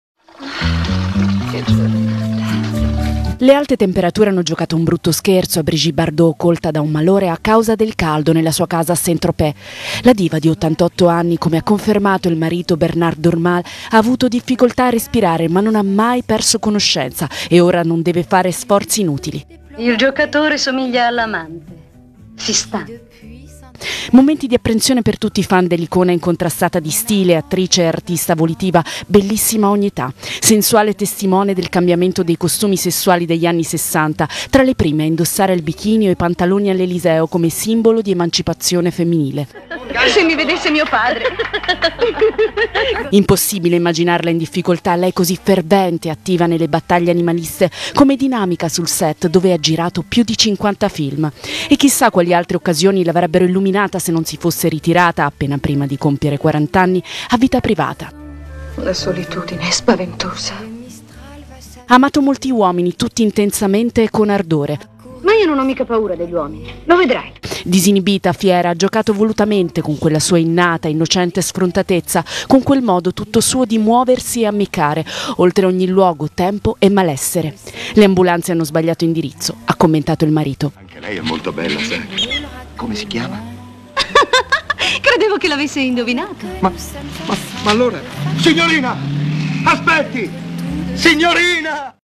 Brigitte Bardot, malore in casa per il caldo
Brigitte Bardot è stata colta da un malore a causa del caldo. I servizi di emergenza sono intervenuti nella casa di Saint-Tropez dell'attrice 88enne. Ecco le sue condizioni.